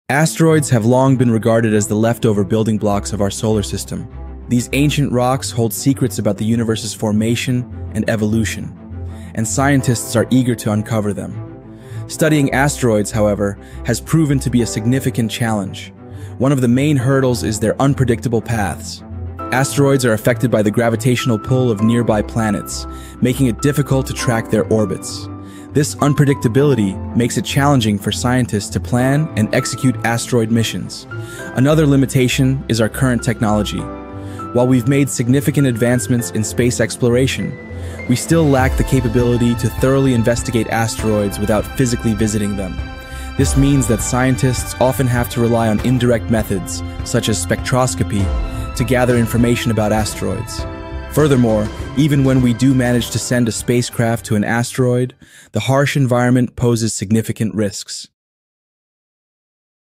ASTEROIDS Hold Secrets To UNIVERSE'S ORIGINS?
Explore the mysterious world of asteroids, remnants from the dawn of our universe, and uncover the secrets they hold about the origins of our cosmos. From the asteroid belt to distant celestial bodies, these ancient rocks have witnessed the birth and evolution of our solar system. Are asteroids the key to understanding the universe's origins? Let's find out!